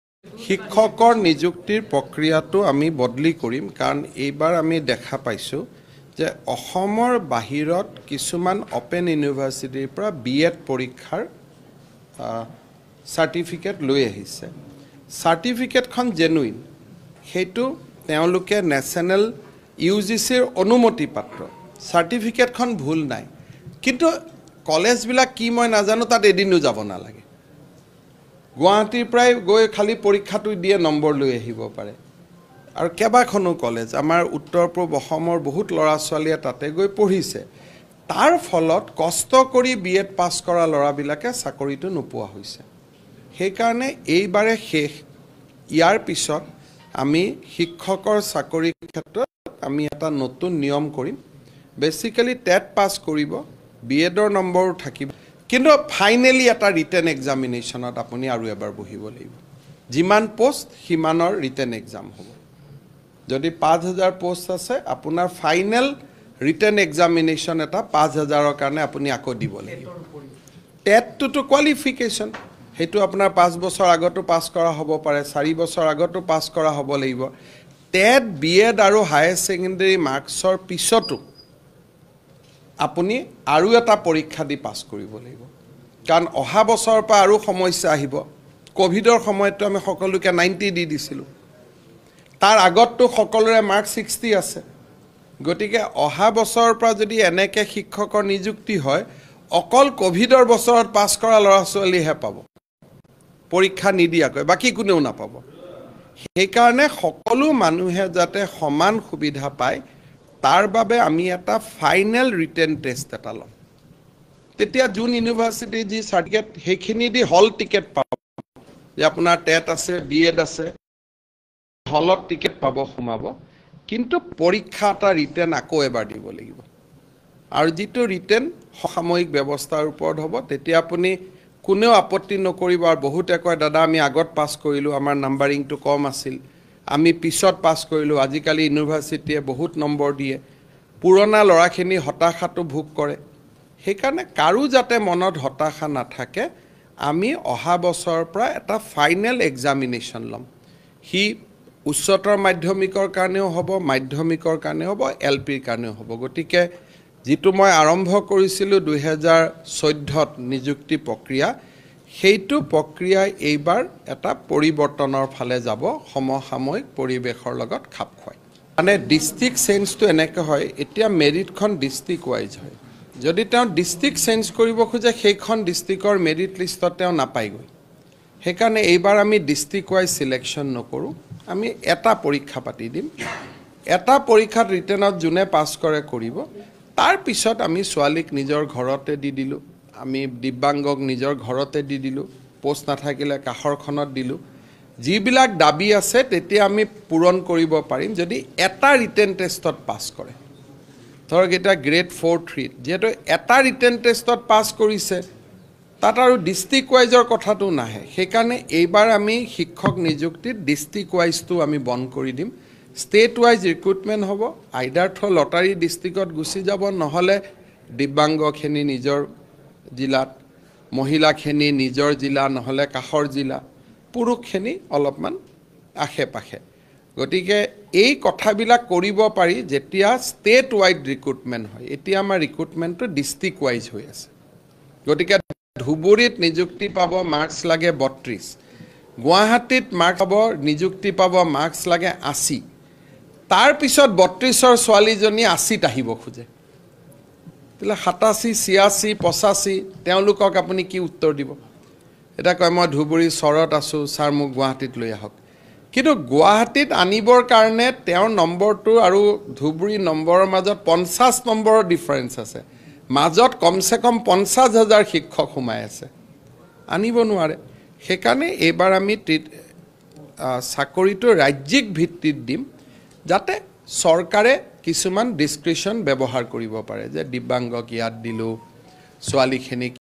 এতিয়াৰে পৰা BEd D El Ed আৰু টেট উত্তীৰ্ণ হ'লেও নাপাব শিক্ষকৰ চাকৰি। ঘোষণা মুখ্যমন্ত্ৰীৰ
এতিয়াৰে পৰা BEd D El Ed আৰু টেট উত্তীৰ্ণ হ'লেও নাপাব শিক্ষকৰ চাকৰি। টেট পাছ কৰাৰ পিছত আকৌ দিব লাগিব পৰীক্ষা। ঘোষণা মুখ্যমন্ত্ৰীৰ